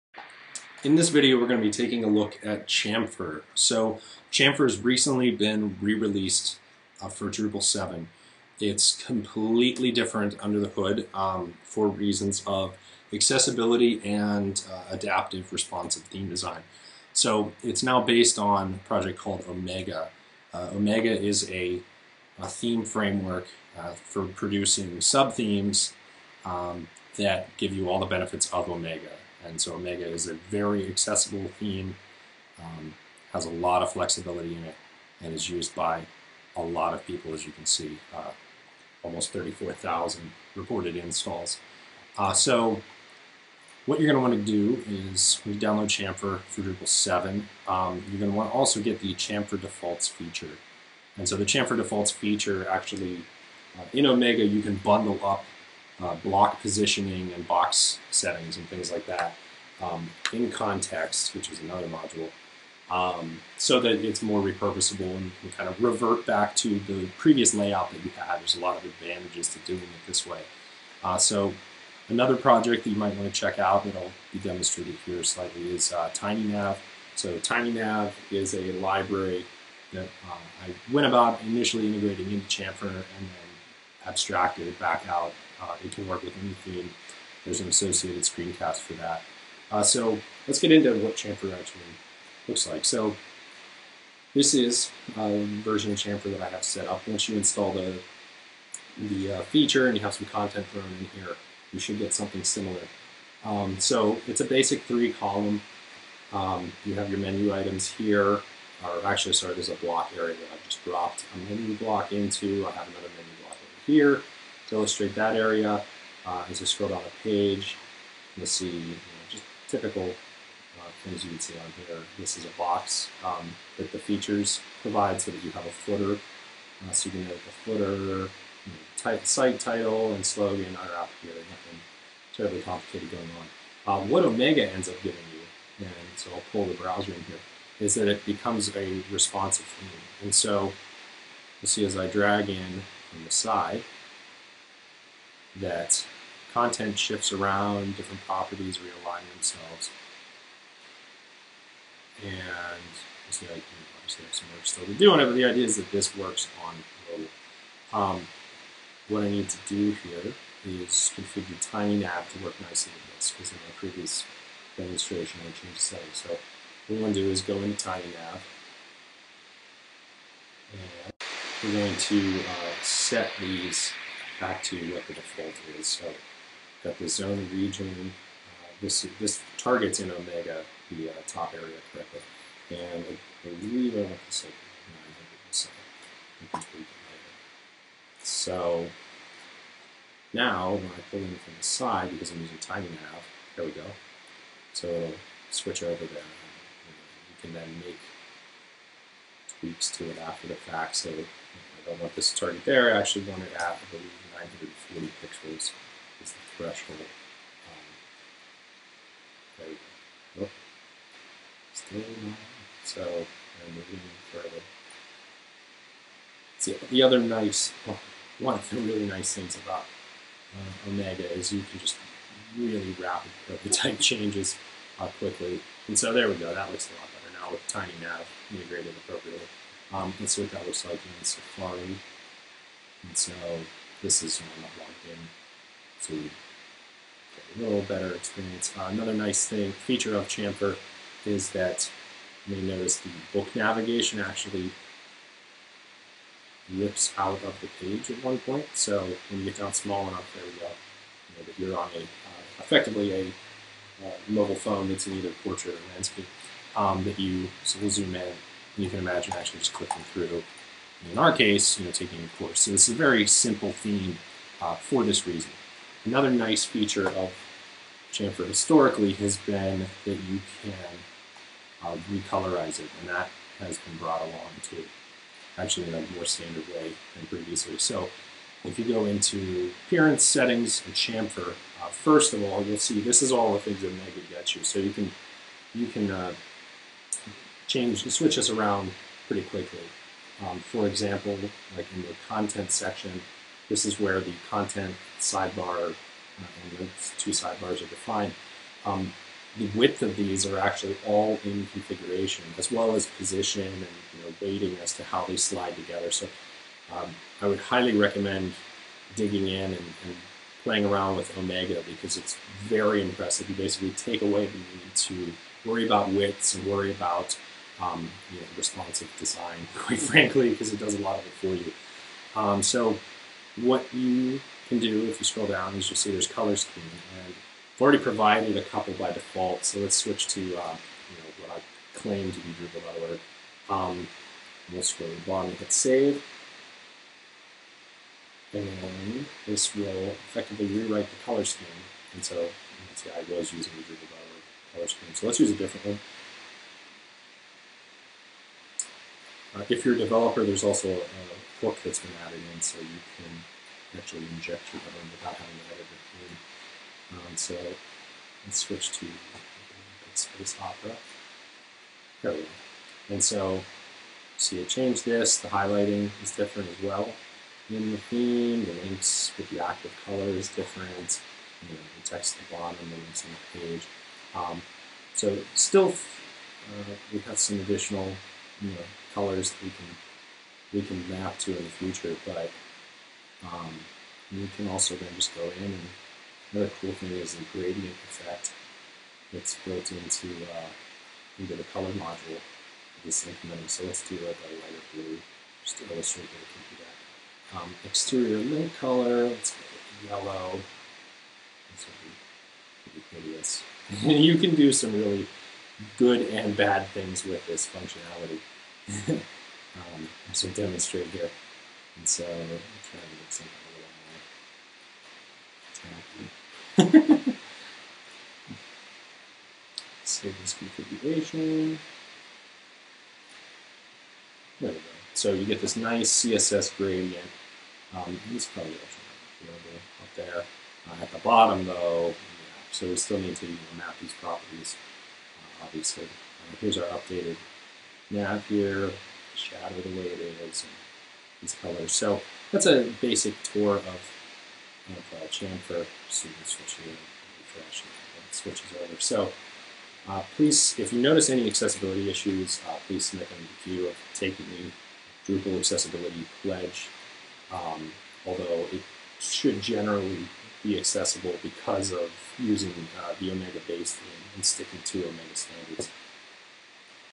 Welcome to Chamfer in Drupal 7
This screencast shows off some of the functionality of Chamfer and how it leverages additional community projects like the color module, tinynav and omega to create better experiences for users of any device.